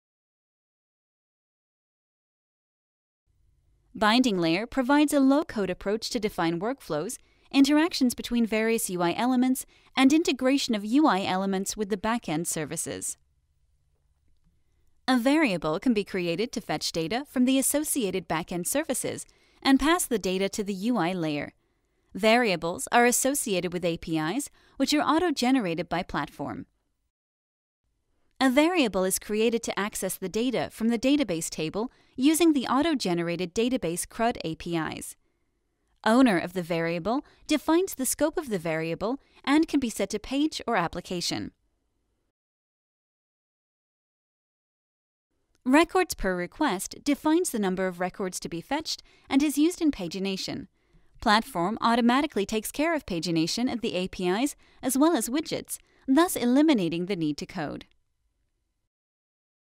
How it Works - API-driven, Low-code Integration
Learn how the middle layer, the binding layer integrates UI elements with the data from the backend services.